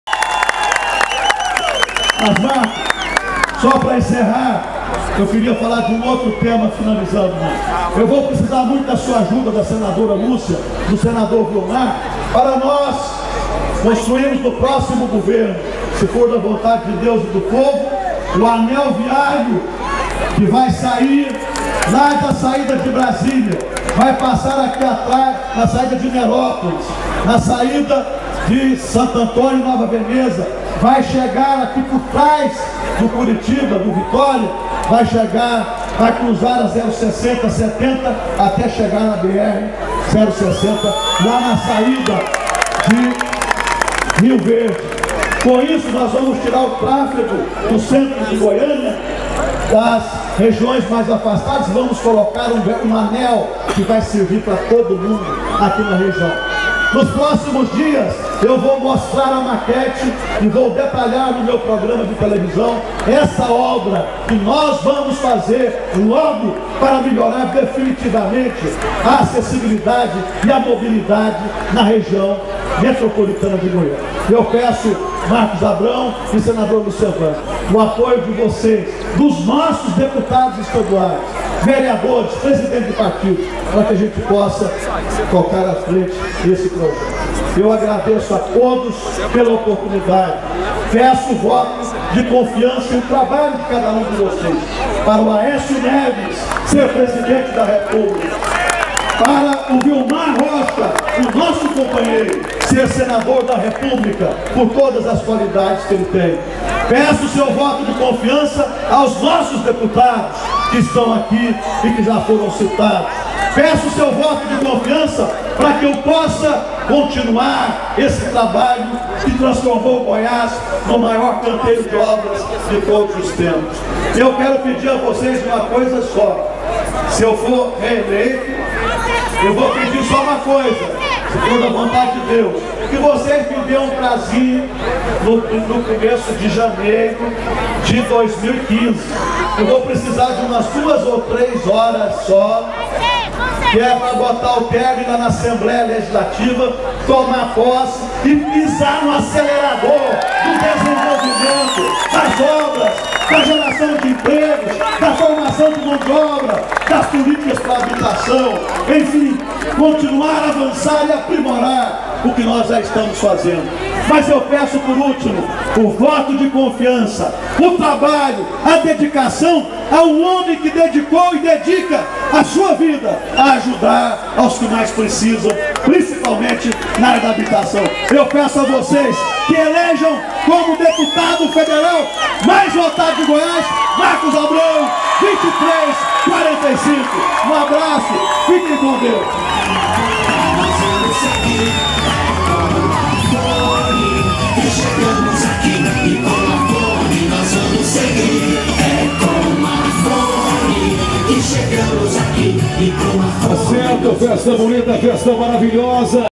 Inauguração de Comitê na Região Noroeste com Marconi Perillo e Marcos Abrão
Presença do prefeito de Palmeiras de Goiás Alberane Marques.